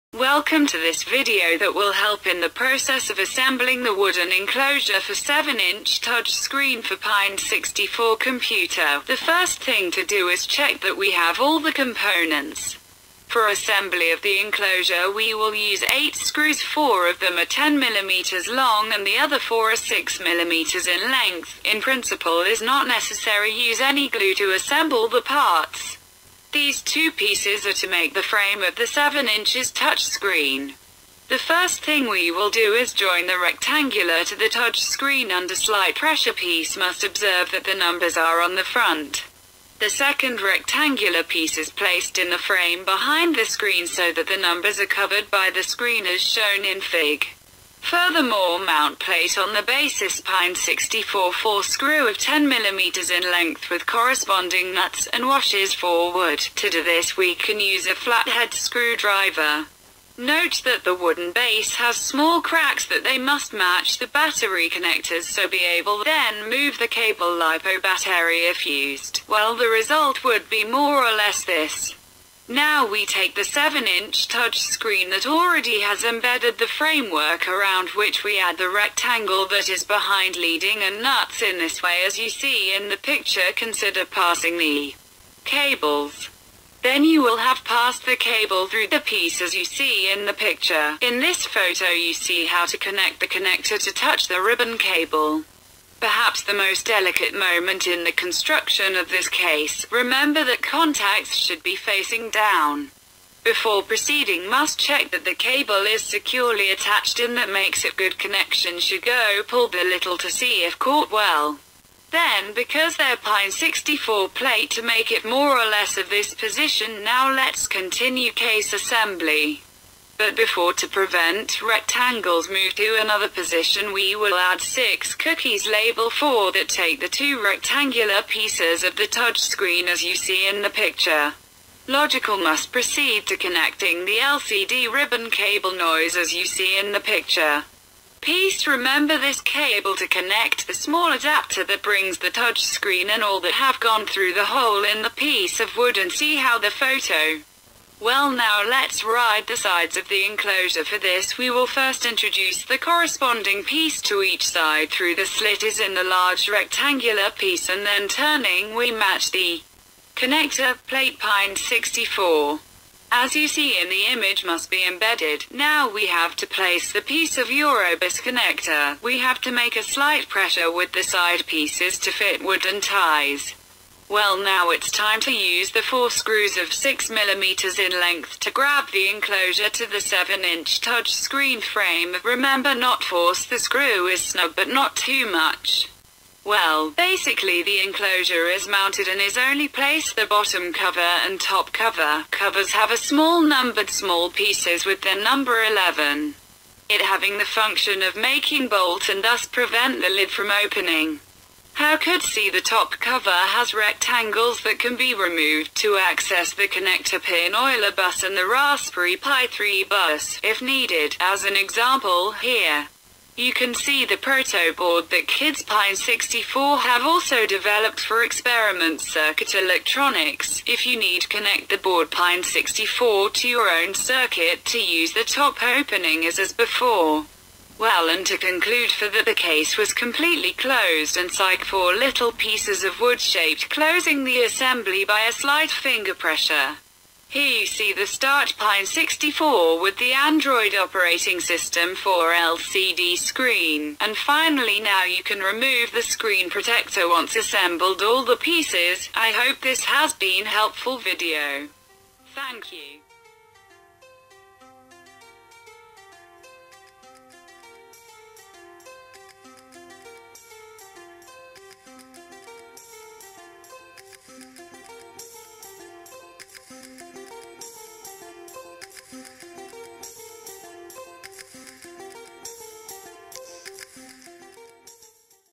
How to assemble the wooden frame enclosure for 7 inch LCD Touch Screen Panel of Pine 64 computer.
An explanation of how to assemble the parts of this KIT.

For sale in AMAZON

Ebay,  ETSY, PONOKO,sites...